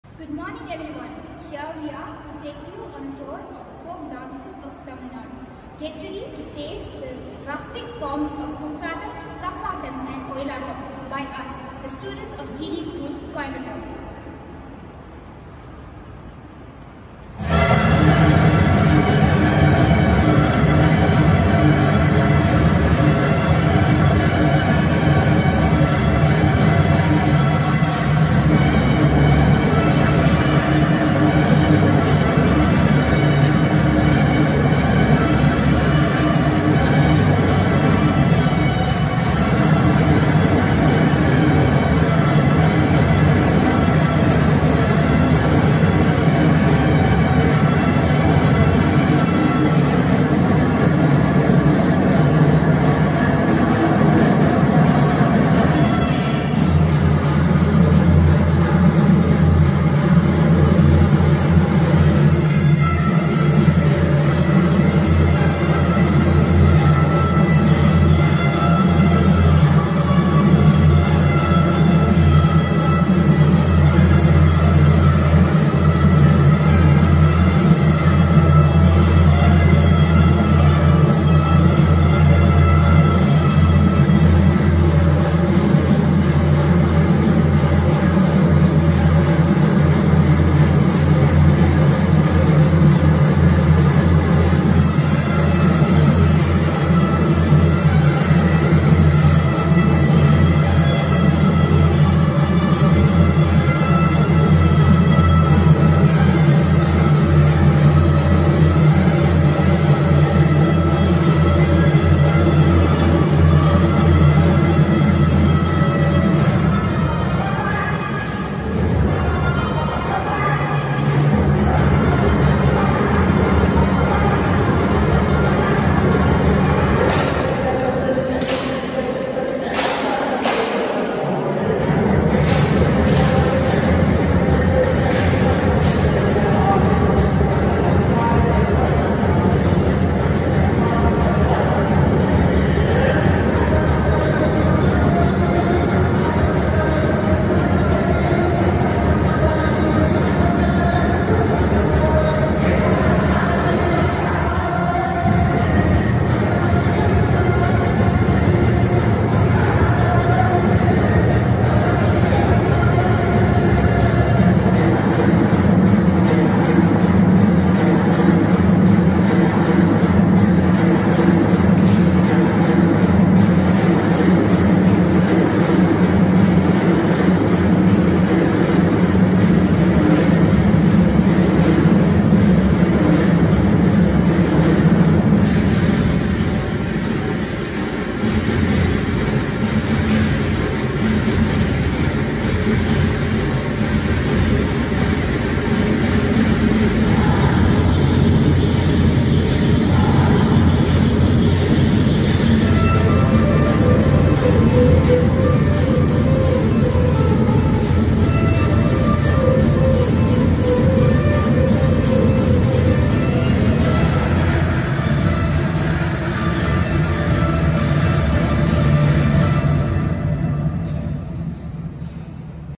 Folk Dance Group | GD-MHSS | Age Category: 17-25 | WFAC - Jhankriti 2022
Our GD Matriculation Higher Secondary School's Submission for Jhankriti2022 WFAC Folk Dance group competition.

Here we present a medley of the folk dances of Tamil Nadu.

Competition: Group Dance - Folk
Age Category: 17 to 25
Age of Students in Video: 17 and 18

Address: GD School, No 2 K K Lane, Old Post Office Road, Coimbatore 641018